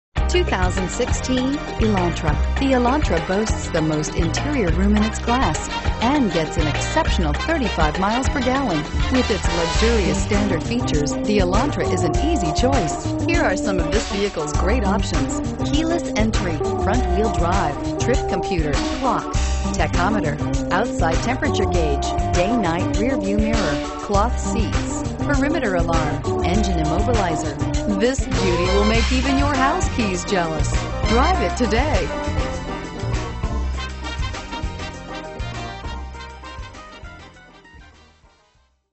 2016 Hyundai Elantra Puyallup WA 337544
2016 Hyundai Elantra GT

2001 N. Meridian

Pick up this Black Noir Pearl 2016 Hyundai Elantra, available today at Larson Automotive Group. Featuring Automatic transmission, it has  miles. This could be the one you've been searching for.